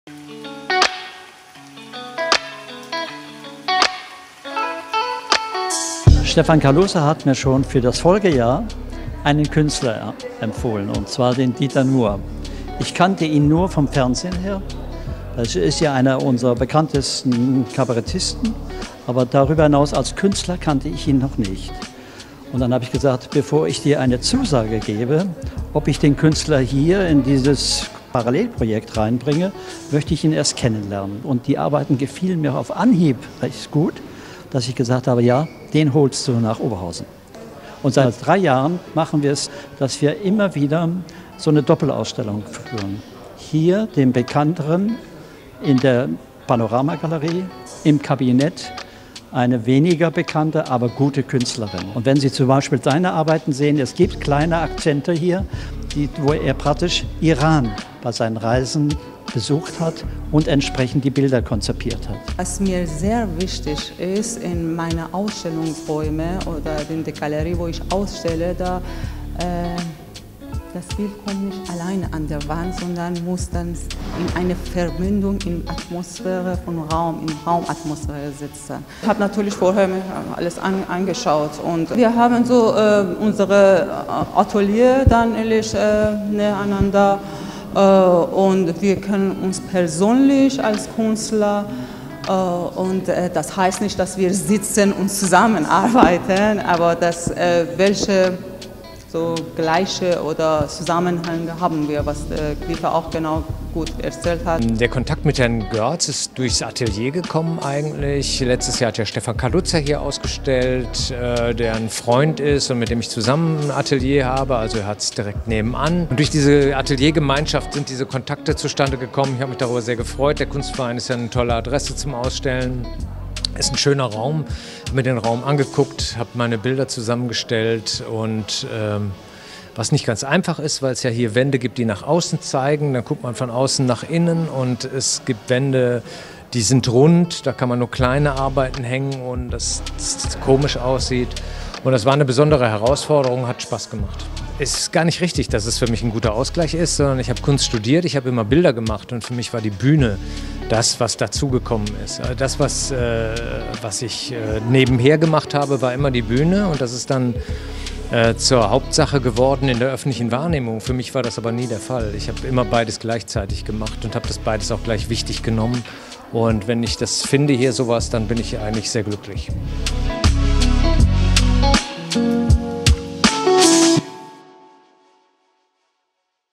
Ludwiggalerie Schloss Oberhausen Dieter Nuhr
Der Kunstverein Oberhausen e.V. hat in der Ludwiggalerie Schloss Oberhausen eine neue Ausstellung eröffnet. In dem Parallelprojekt zeigt nicht nur die Künstlerin Bahar Batvand ihre Werke, sondern auch Künstler und Kabarettist Dieter Nuhr.